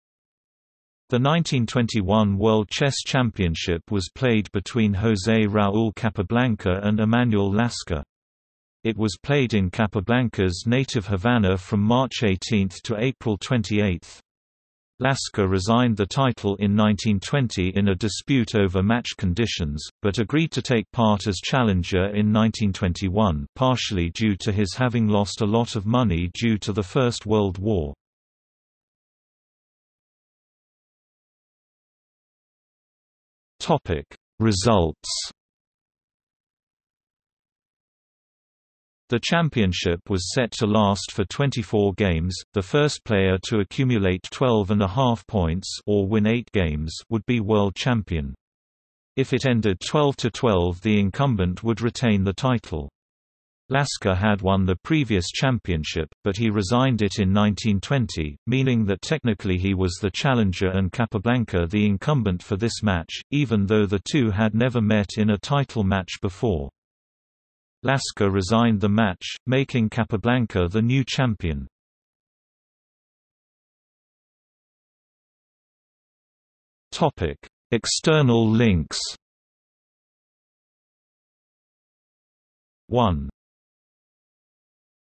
World Chess Championship 1921 | Wikipedia audio article
00:00:30 Results
00:01:13 External links

- Socrates

SUMMARY
The 1921 World Chess Championship was played between José Raúl Capablanca and Emanuel Lasker. It was played in Capablanca's native Havana from March 18 to April 28.  Lasker resigned the title in 1920 in a dispute over match conditions, but agreed to take part as challenger in 1921 (partially due to his having lost a lot of money due to the First World War).